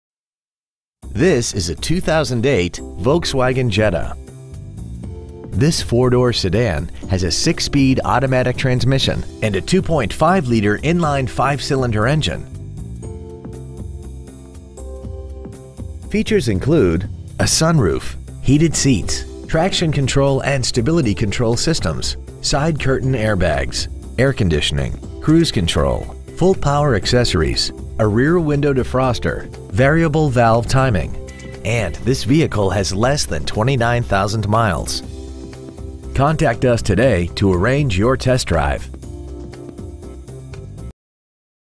2008 Volkswagen Jetta Highland Park IL
We have been honored to serve the Highland Park IL area , we promise that your experience at our dealership will exceed your expectations ! Year : 2008 Make : Volkswagen Model : Jetta Engine : 2.5L 5 cyls Trans . : Automatic 6-Speed Exterior : Reflex Silver Metallic Miles : 28,858 Interior : Art Gray w/V-Tex Leatherette Seating Surfaces Stock : H29530A Muller Honda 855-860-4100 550 Skokie Valley Road Highland Park , IL 60035